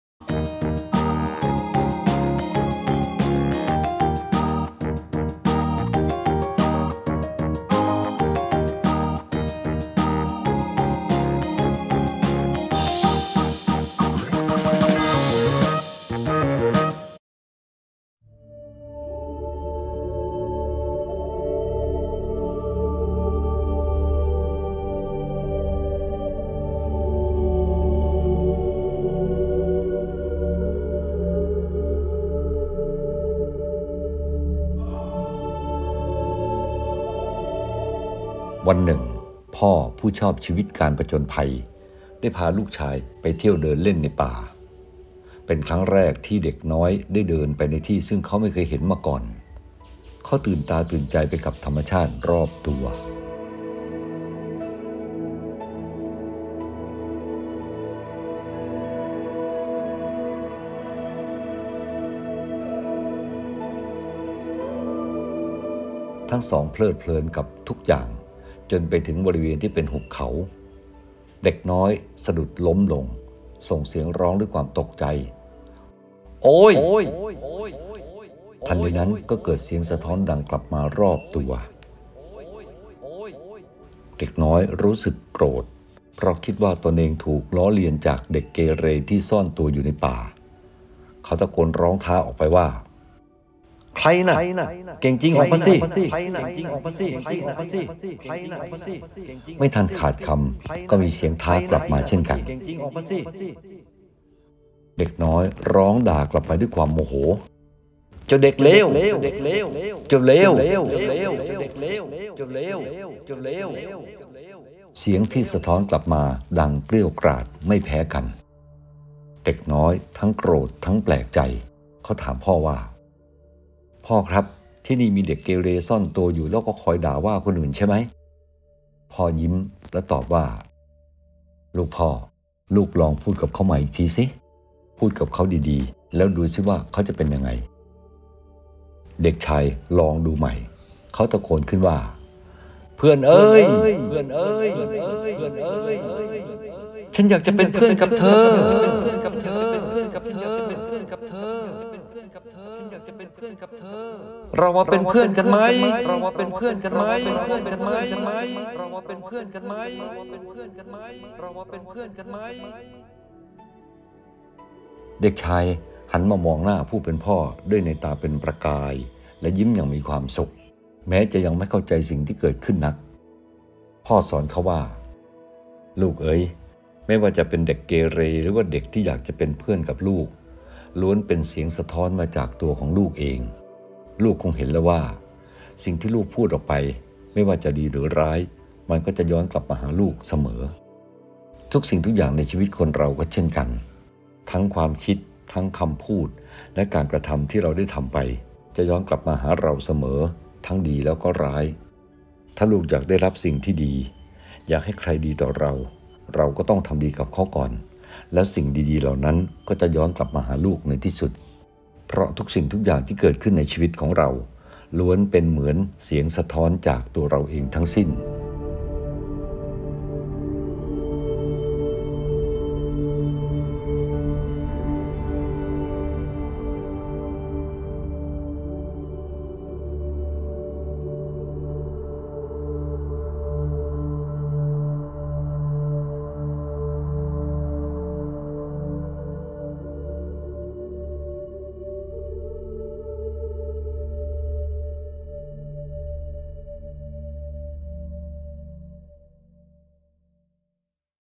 เสียงลึกลับจากหุบเขา | เรื่องเล่ารับอรุณ เรื่องที่ 346 | รุ้งแปดสี
เสียงลึกลับนั้นดังมาจากหุบเขา
ตอนที่ลูกชายไปเดินป่ากับพ่อ
འ
ต้อนรับวันใหม่ทุกวัน เวลา 05.00 น.
กับเรื่องราวและมุมคิดเสริมพลังชีวิต